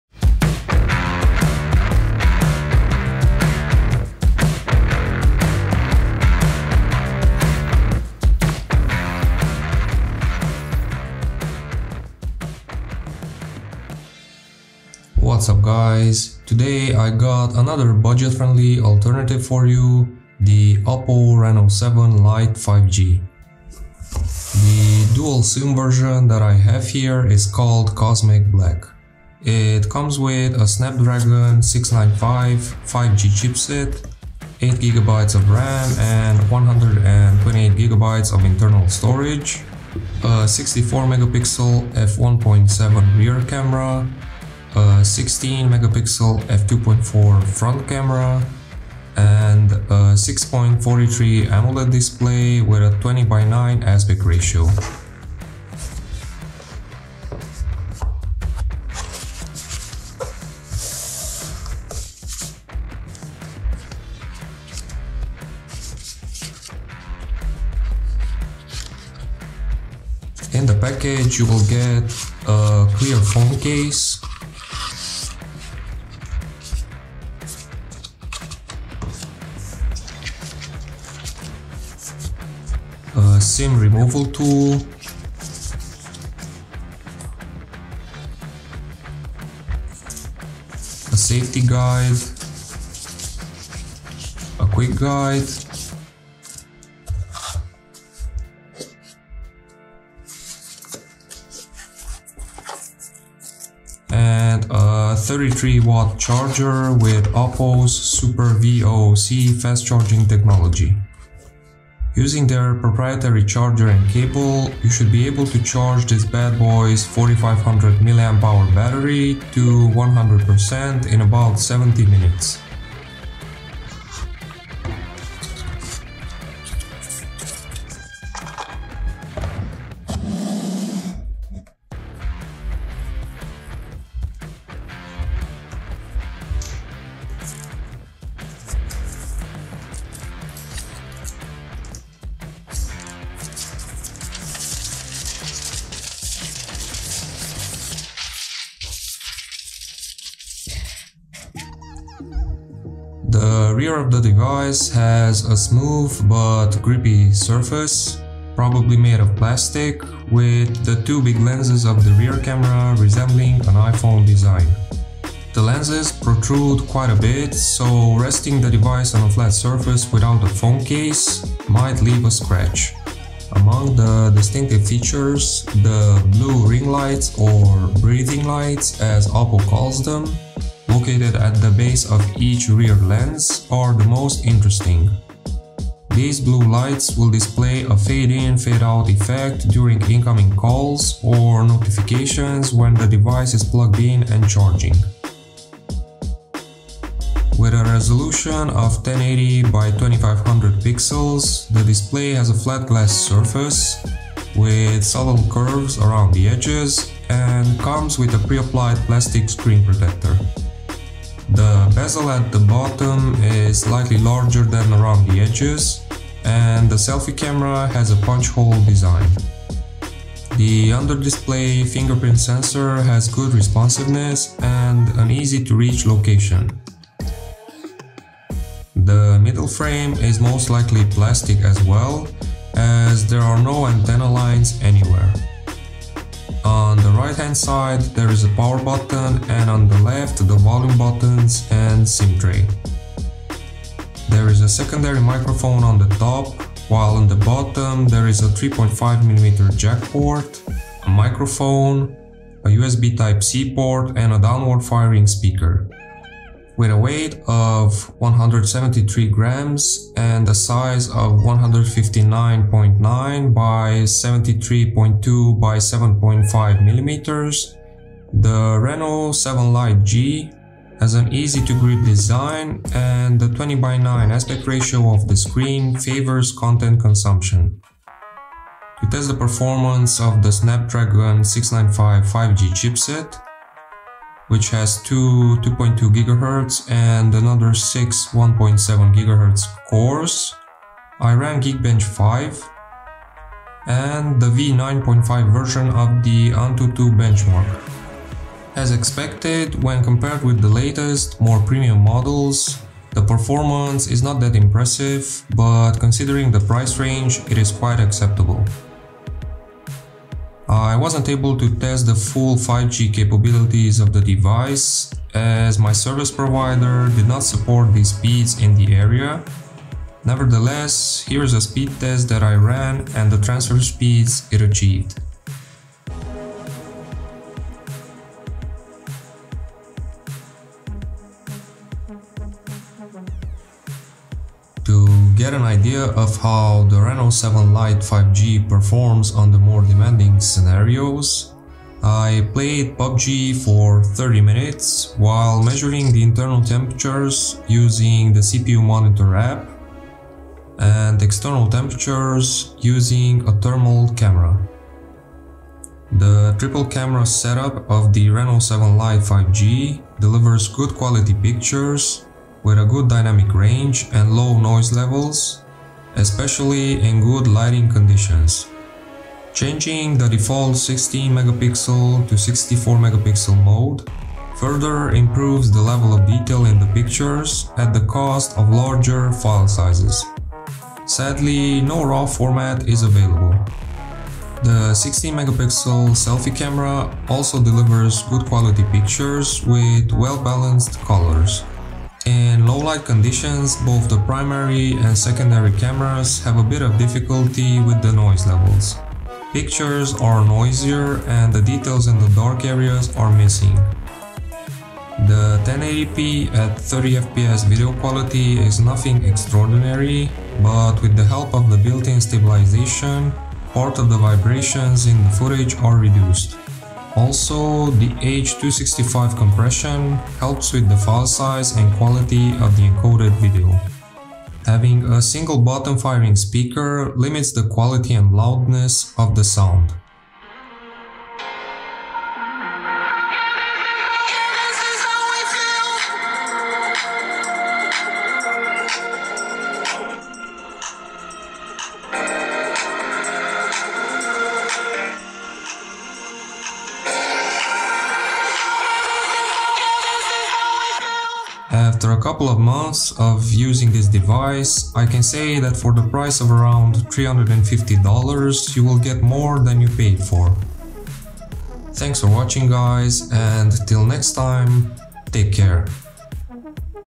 Some thoughts on the OPPO Reno 7 Lite 5G
Another well-performing, budget-friendly device, the OPPO Reno 7 Lite 5G has good battery life and will do the job in daily use.

Chapters:
0:00 Intro
0:24 Unboxing
2:32 Design and Specs
4:38 CPU Performance
5:35 Gaming Performance
5:57 Camera Performance
7:05 Speaker Performance
7:31 Conclusion